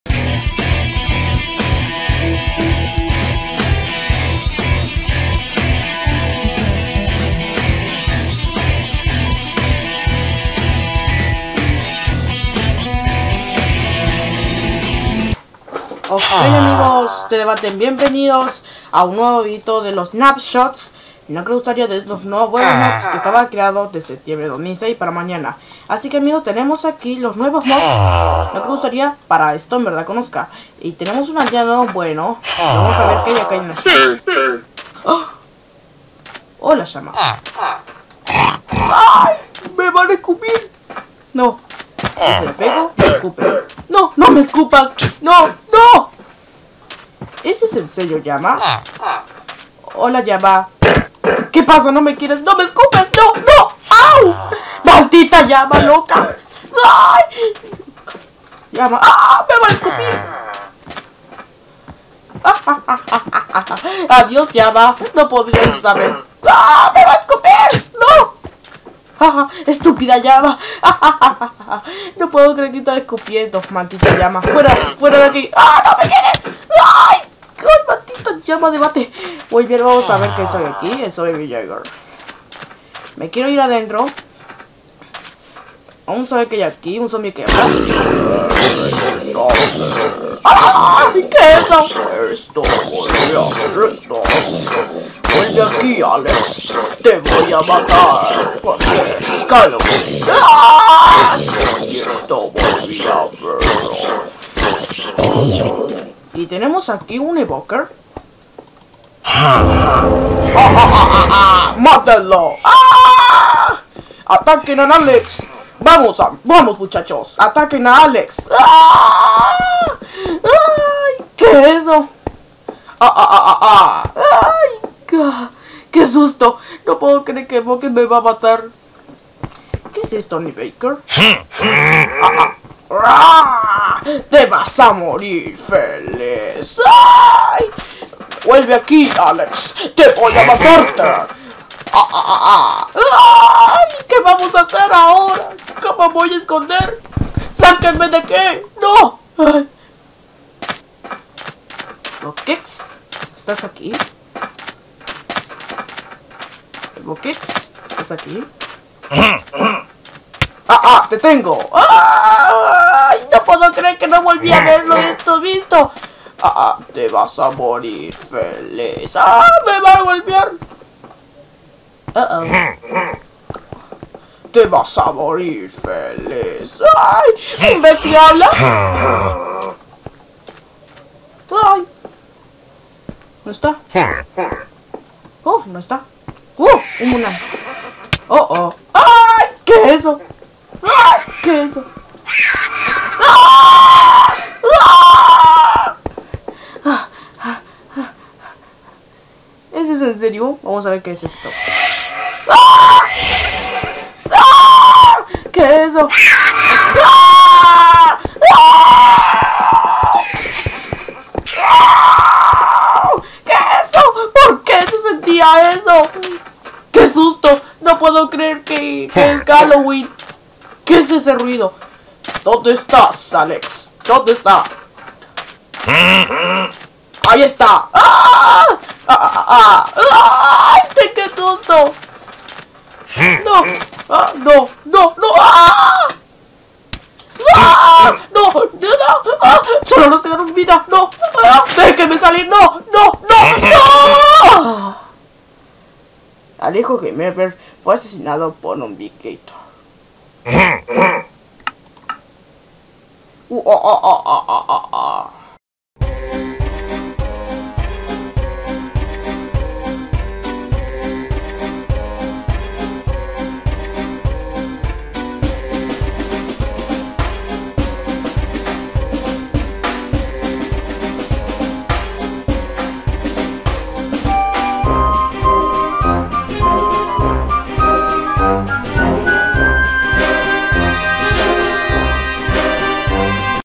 nuevos mobs de septiembre 2016 especial halloween 2016
alex celebra halloween de verdar con los nuevo mobs que van atacando antes